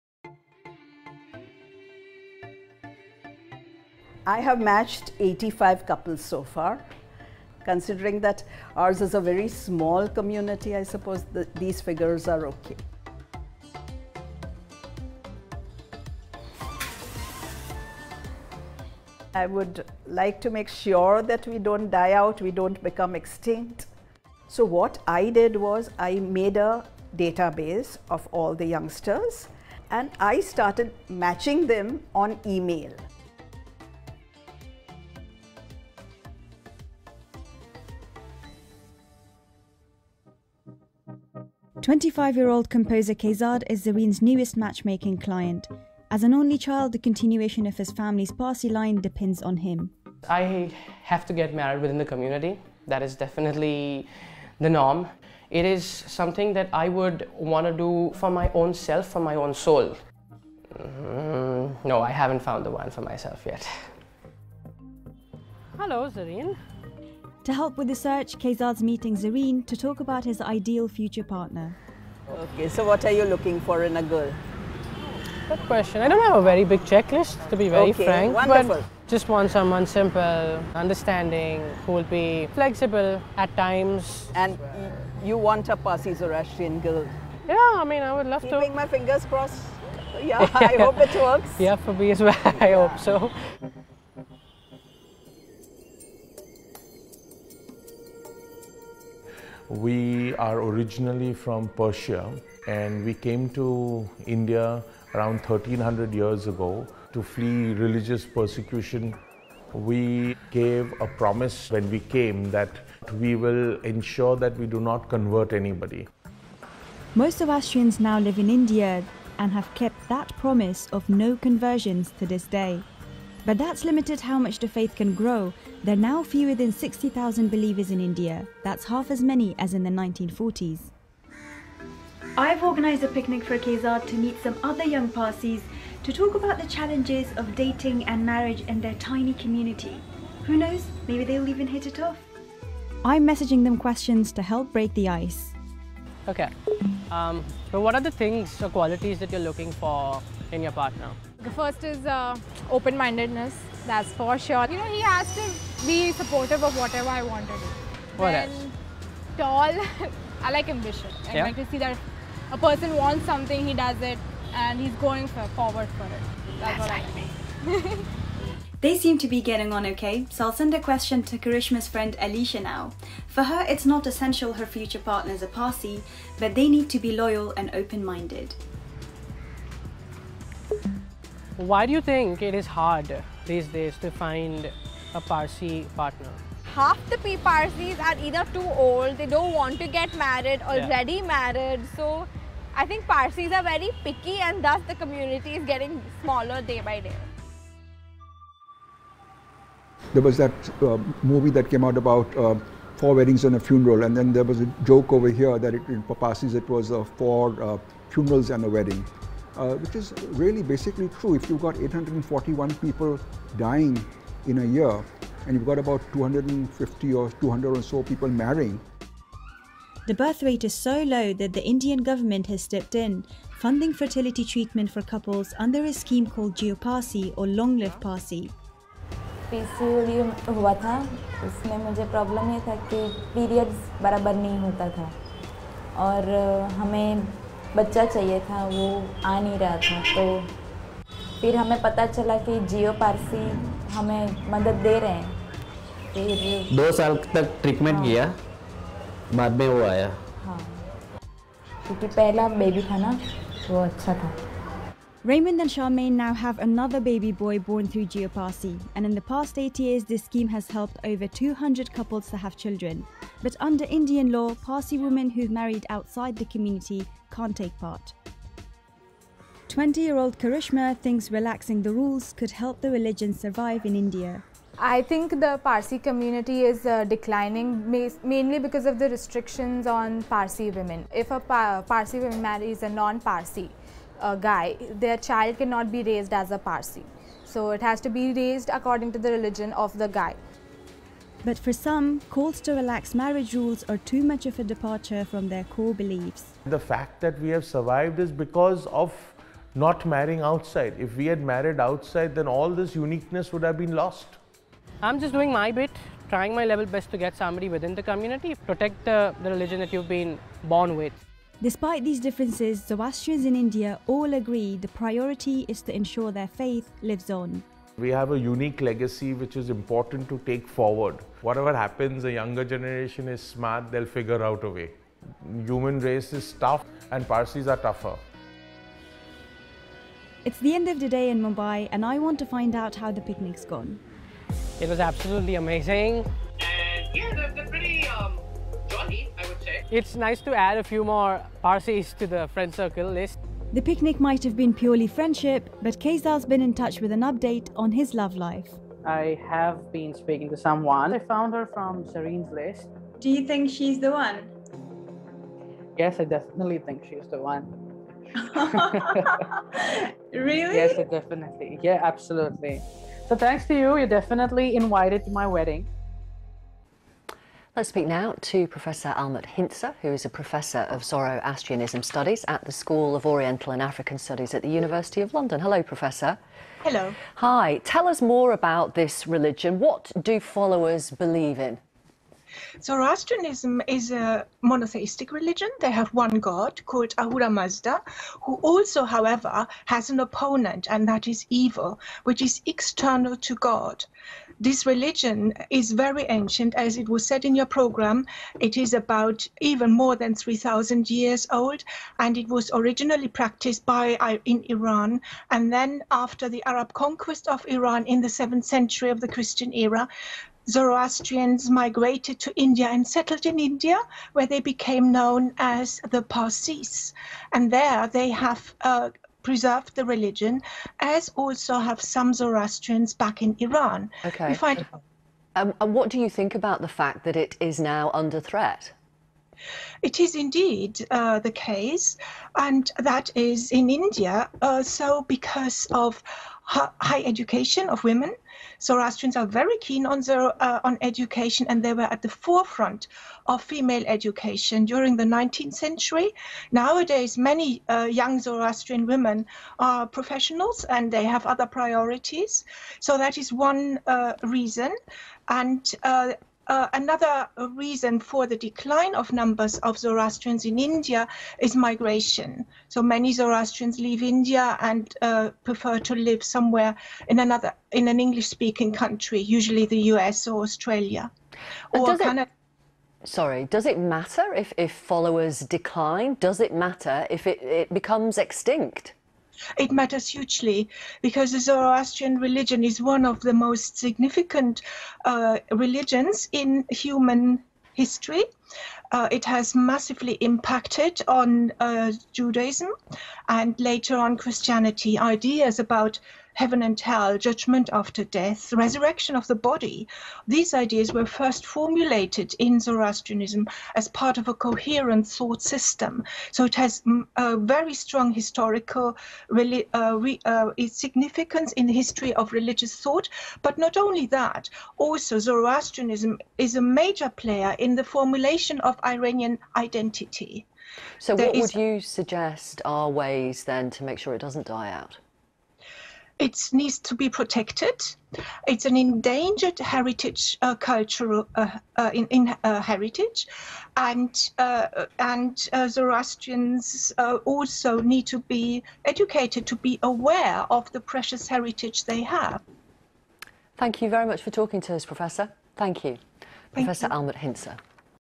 BBC Documentary on the Jiyo Parsi Program, March 2021
The full BBC segment on the Jiyo Parsi program including interviews with Dr. Almut Hintze and Xerxes Dastur